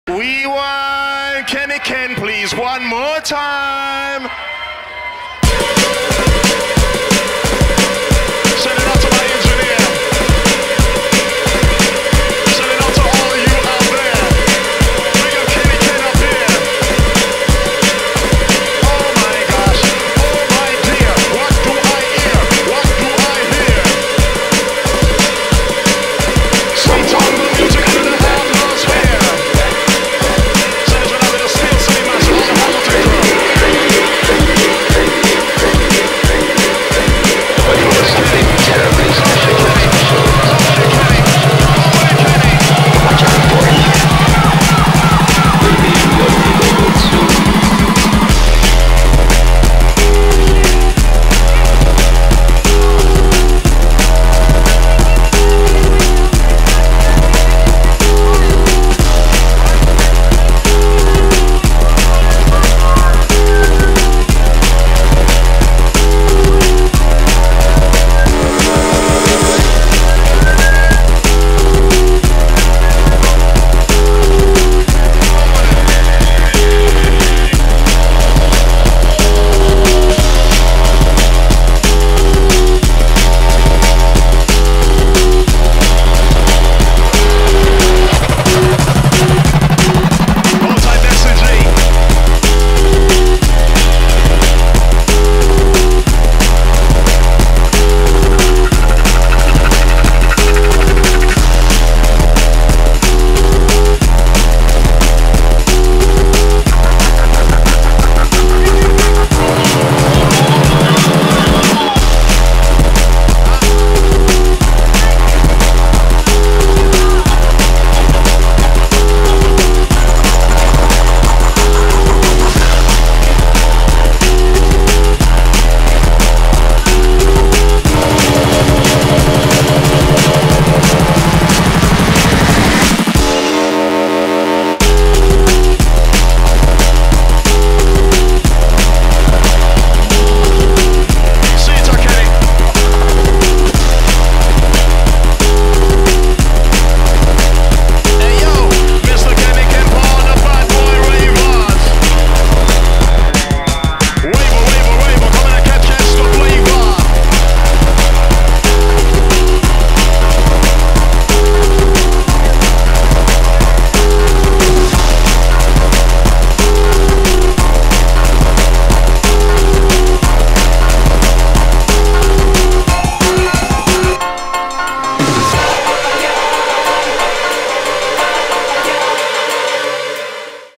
Kenny Ken Live At 'Kings Of The Jungle' Toronto
A live scene from Kings Of The Jungle from Toronto in 2006. DJ Kenny Ken kills it in Sound Academy.